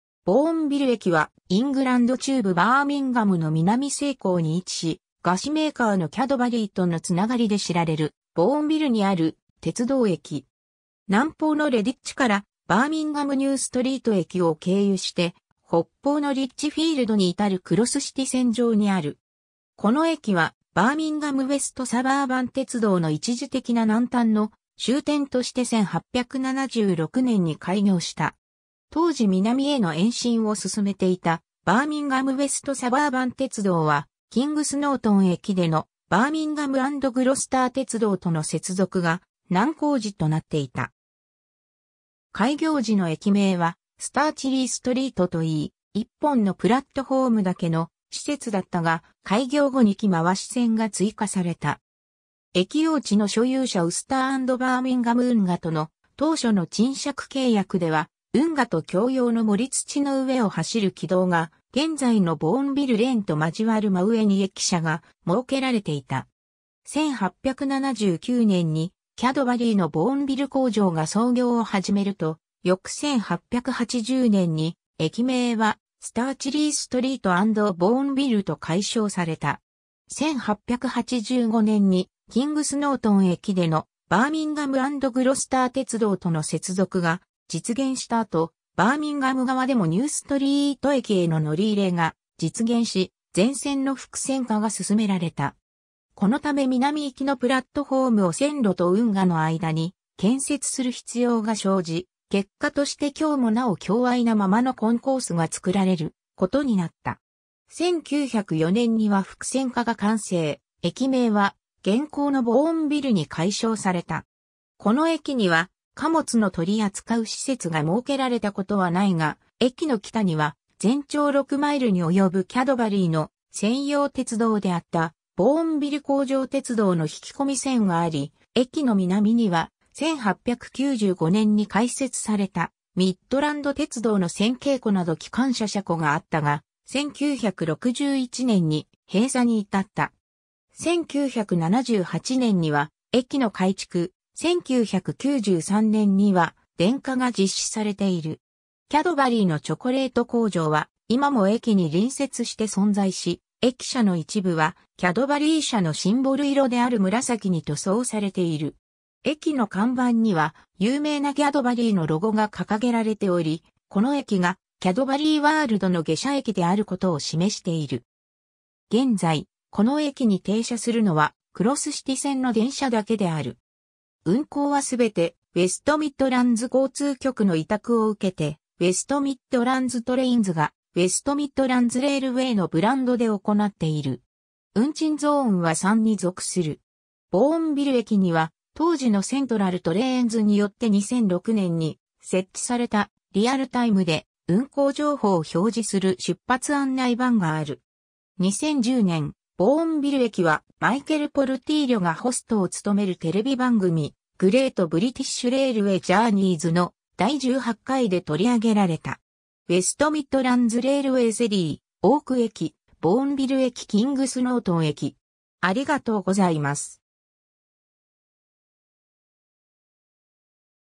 ボーンビル駅
ボーンビル駅（ボーンビルえき、英語:Bournville station）は、イングランド中部バーミンガムの南西郊に位置し、菓子メーカーのキャドバリーとのつながりで知られるボーンビルにある鉄道駅。
南方のレディッチから、バーミンガム・ニューストリート駅を経由して北方のリッチフィールドに至るクロス・シティ線上にある。
 この駅はバーミンガム・ウェスト・サバーバン鉄道の一時的な南端の終点として1876年に開業した。
当時南への延伸を進めていたバーミンガム・ウェスト・サバーバン鉄道は、キングス・ノートン駅でのバーミンガム・ アンド・グロスター鉄道との接続が難工事となっていた。
開業時の駅名はスターチリー・ストリート (Stirchley Street) といい、一本のプラットホームだけの施設だったが、開業後に機回し線が追加された。
駅用地の所有者ウスター・アンド・バーミンガム運河との当初の賃借契約では、運河と共用の盛土の上を走る軌道が現在のボーンビル・レーン (Bournville Lane) と交わる真上に駅舎が設けられていた。
 1879年にキャドバリーのボーンビル工場が操業を始めると、翌1880年に駅名はスターチリー・ストリート・アンド・ボーンビル (Stirchley Street and Bournville) と改称された。
1885年にキングス・ノートン駅でのバーミンガム・ アンド・グロスター鉄道との接続が実現した後、バーミンガム側でもニューストリート駅への乗り入れが実現し、全線の複線化が進められた。
このため南行きのプラットホームを線路と運河の間に建設する必要が生じ、結果として今日もなお狭隘なままのコンコースが作られることになった。
1904年には複線化が完成、駅名は現行のボーンビルに改称された。
 この駅には貨物の取り扱う施設が設けられたことはないが、駅の北には、全長6マイル（およそ10キロメートル）に及ぶキャドバリーの専用鉄道であったボーンビル工場鉄道 (Bournville Works Railway) の引き込み線があり、駅の南には、1895年に開設されたミッドランド鉄道の扇形庫など機関車車庫があったが、1961年に閉鎖に至った。
 1978年には駅の改築、1993年には電化が実施されている。
 キャドバリーのチョコレート工場は今も駅に隣接して存在し、駅舎の一部はキャドバリー社のシンボル色である紫に塗装されている。
駅の看板には、有名なキャドバリーのロゴが掲げられており、この駅がキャドバリーワールドの下車駅であることを示している。
 現在、この駅に停車するのはクロス・シティ線の電車（323形を使用）だけである。
運行はすべて、ウェスト・ミッドランズ交通局の委託を受けて、ウェスト・ミッドランズ・トレインズがウェスト・ミッドランズ・レールウェイのブランドで行っている。
運賃ゾーンは3に属する。
 ボーンビル駅には、当時のセントラル・トレインズによって2006年に設置された、リアルタイムで運行情報を表示する出発案内板がある。
 2010年、ボーンビル駅は、マイケル・ポルティーリョがホストを務めるテレビ番組『Great British Railway Journeys』の第18回で取り上げられた。
 ■ウェスト・ミッドランズ・レールウェイ（ウェスト・ミッドランズ・トレインズ） セリー・オーク駅 - ボーンビル駅 - キングス・ノートン駅